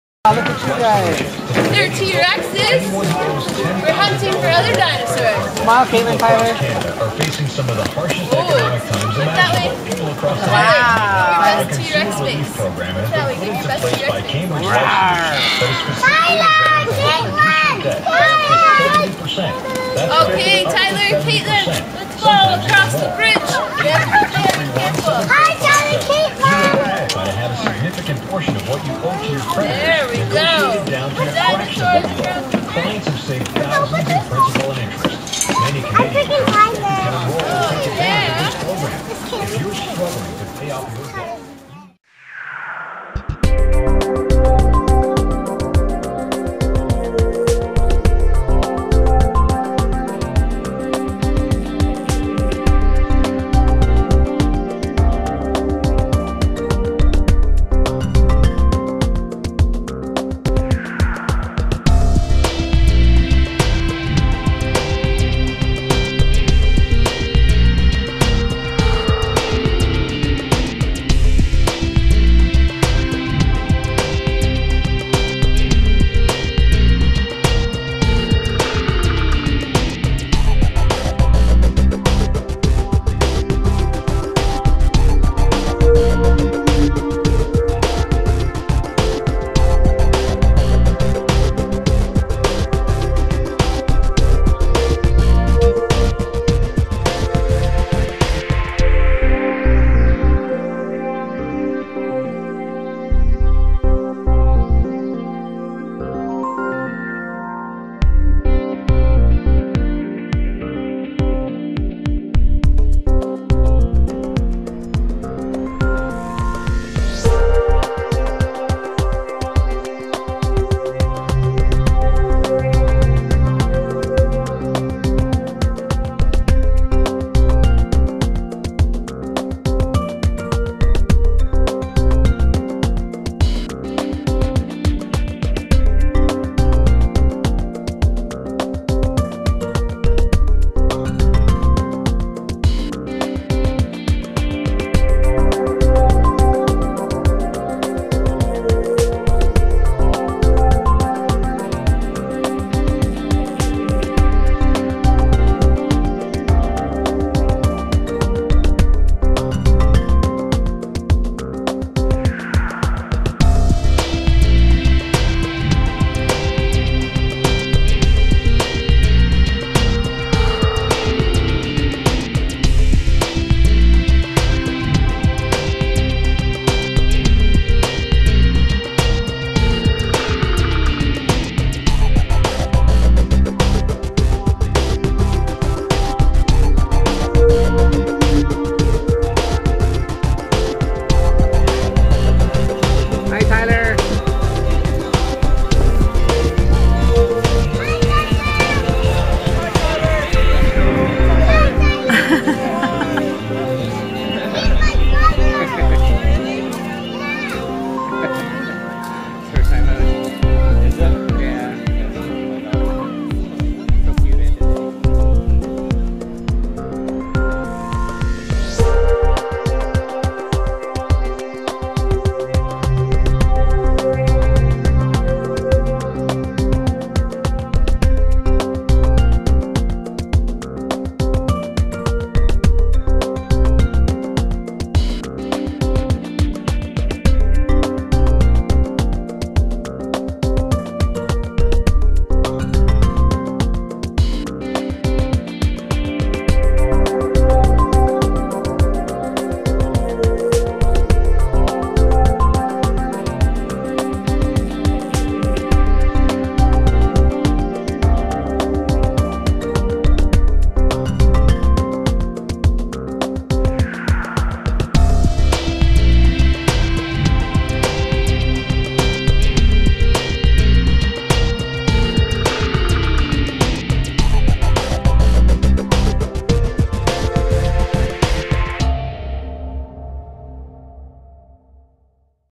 First ski lesson at Blue Mountain
Two three-year-olds taking their first ski lesson at Blue Mountain Resort, Collingwood, Ontario